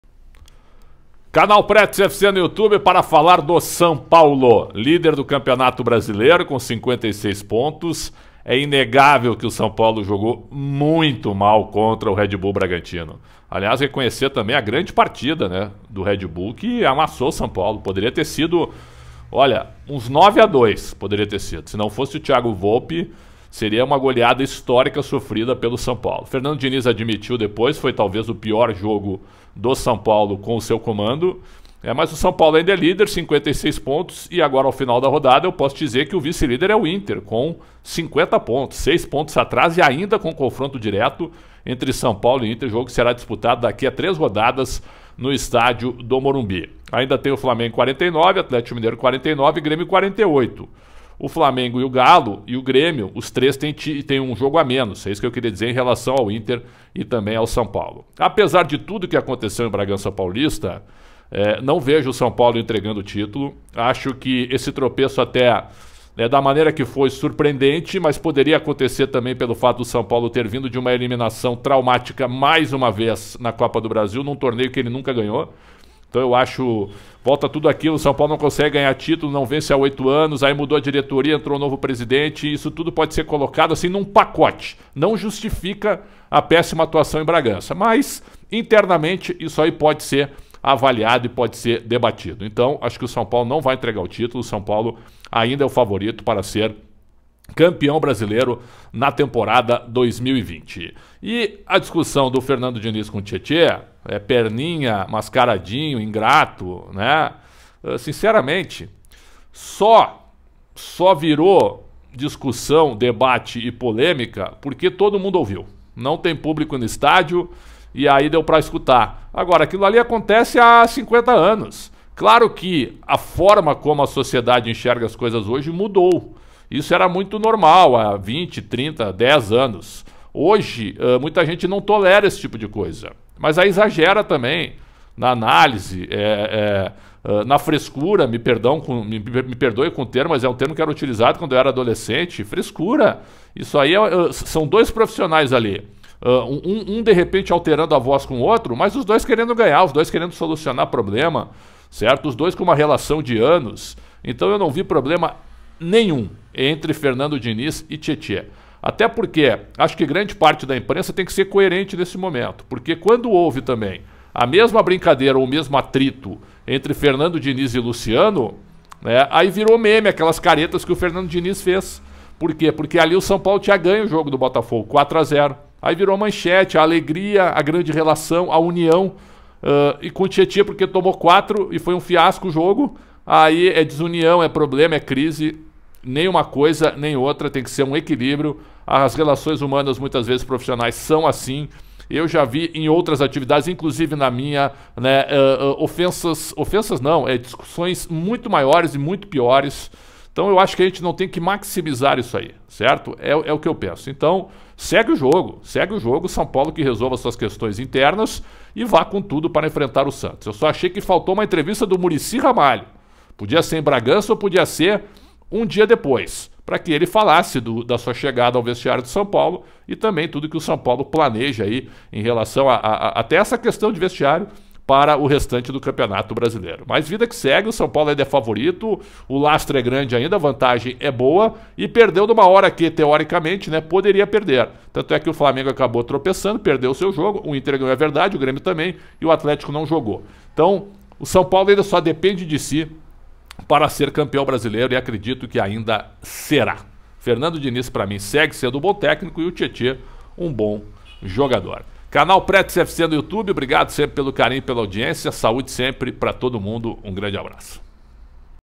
Sigo com a convicção do São Paulo Campeão Brasileiro. Diniz e Tchê Tchê foi algo normal
Tricolor só depende de si e segue como favorito ao título.

Foto; spfc.net